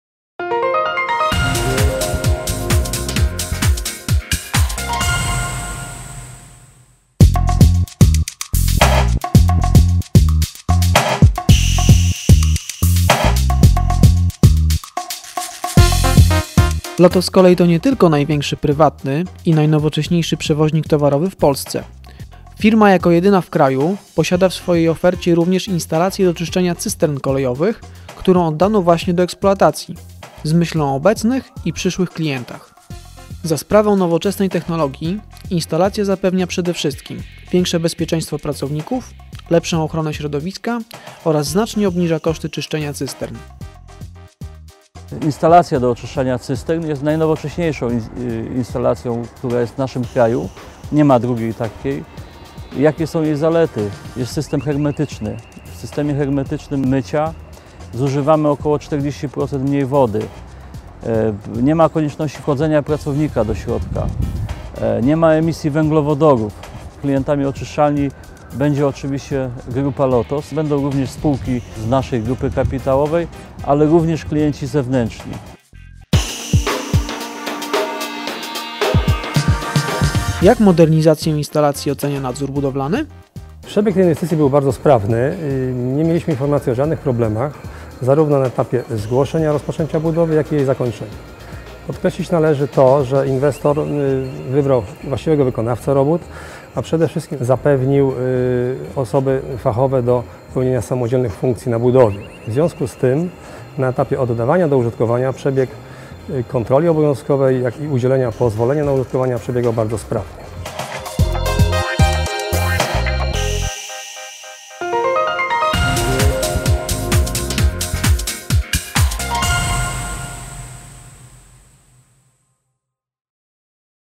LOTOS Kolej ma najnowocześniejszą instalację czyszczenia cystern w Polsce
LOTOS Kolej, największy prywatny przewoźnik towarowy w kraju, uruchomiła  w Gdańsku zmodernizowaną instalację do czyszczenia cystern kolejowych. Dzięki modernizacji LOTOS Kolej znacznie rozszerzyła zakres świadczonych usług.

-- Wszystko po to by przedstawić naszym Klientom jeszcze bardziej kompletną i konkurencyjną ofertę -- podkreśla Katarzyna Korowicka, dyrektor ds. handlu LOTOS Kolej. -- Już dziś oferujemy szeroki asortyment usług. Do zadań z zakresu spedycji, przewozów czy wynajmu i serwisu technicznego wagonów doszło teraz czyszczenie cystern. Co ważniejsze konkurujemy jakością, stawiając na bezpieczeństwo, nowoczesność i terminowość. Wybór pozostawiamy Klientom.